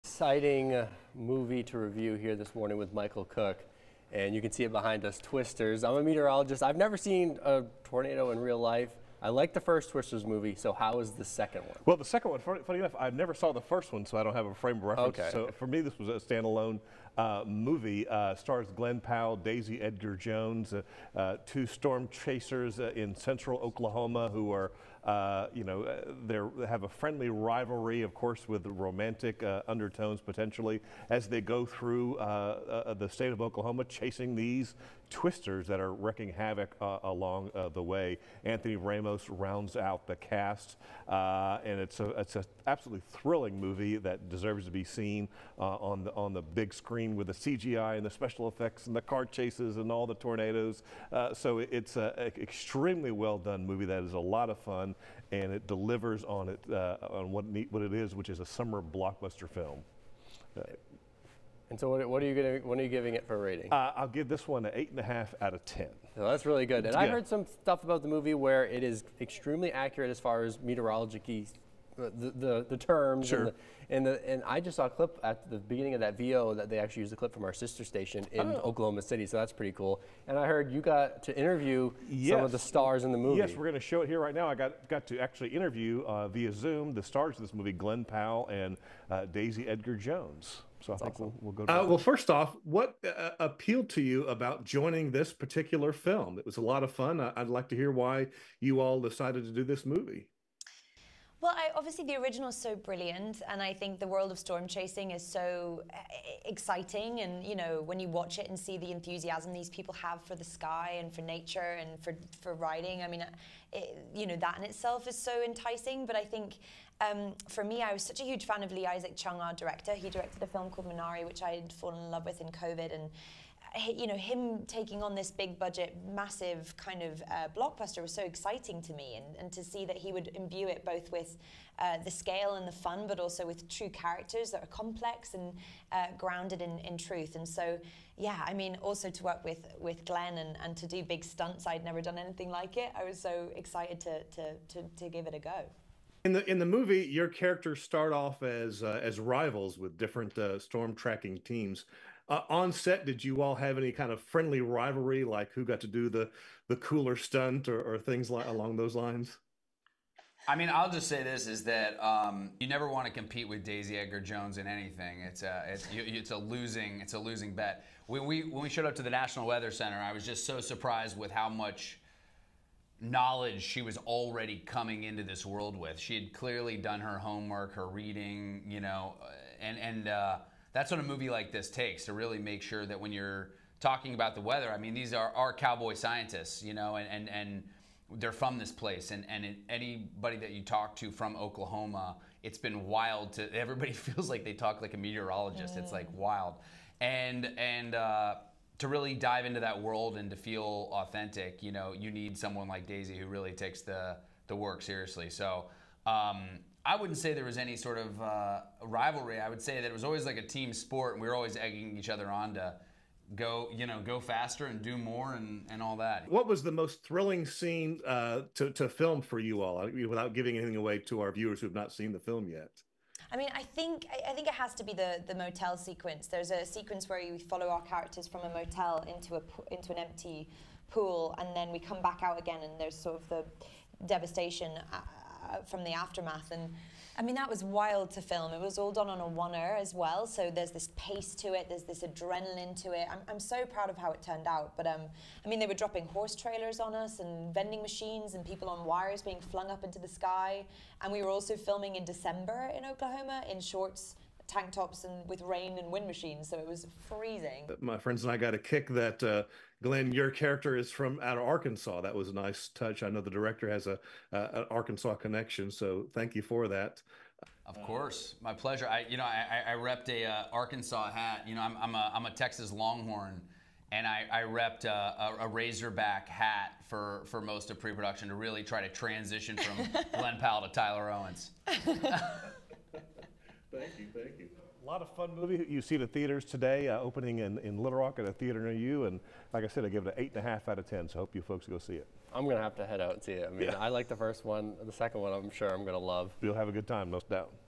Interview with Twisters stars Glen Powell & Daisy Edgar-Jones.
I interviewed Twisters stars Glen Powell & Daisy Edgar-Jones for KLRT-TV in Little Rock.